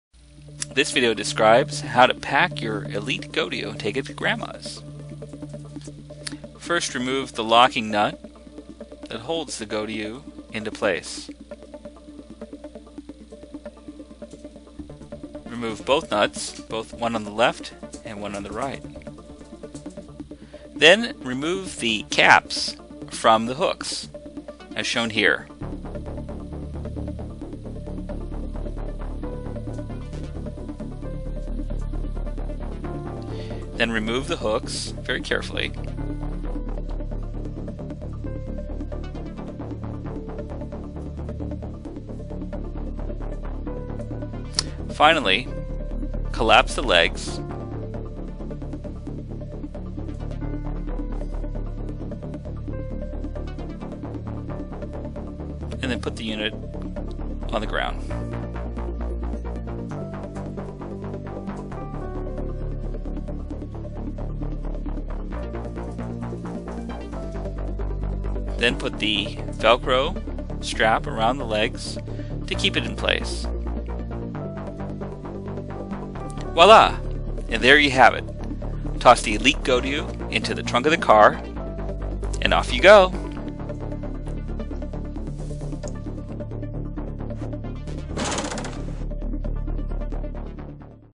Elite Ghodiyu
How to stow your Elite Ghodiyu and pack it into your car.